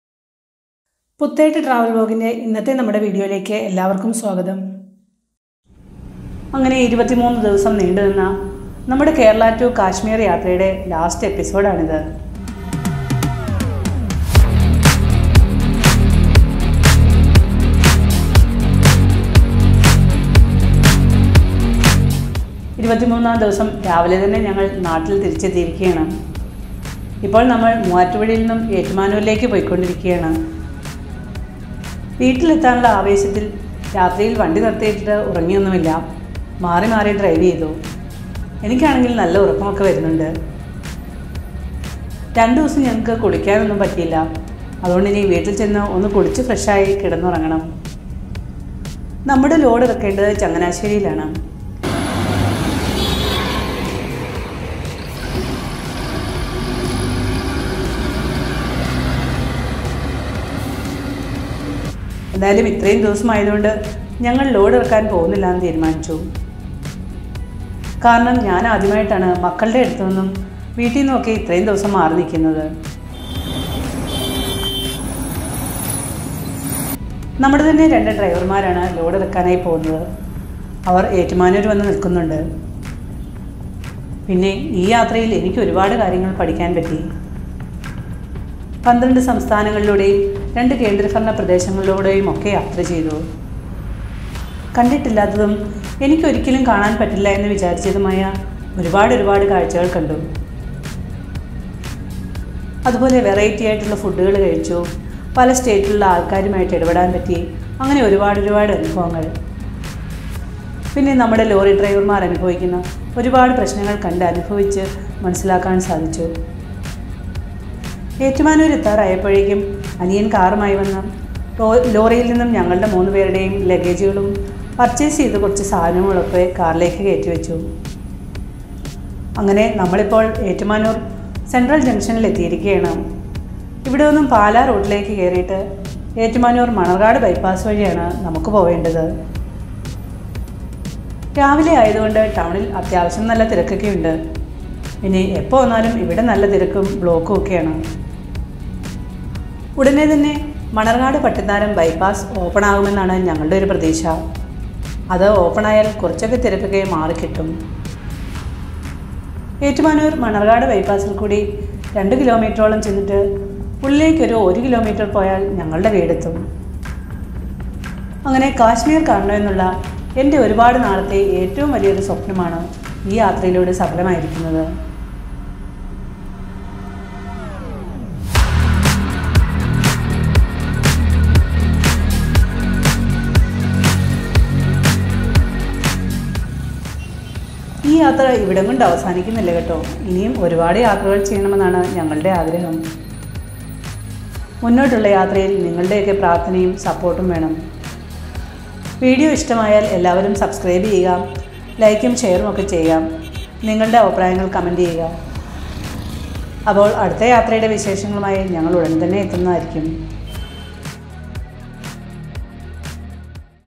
23 ദിവസത്തിന് ശേഷം, നാട്ടിൽ തിരിച്ചെത്തി | Part -15 | Day 23 | KASHMIR (Jammu & Kashmir)
23 ദിവസത്തിന് ശേഷം, നാട്ടിൽ തിരിച്ചെത്തി
Puthettu Travel Vlog  - Malayalam Travel vlog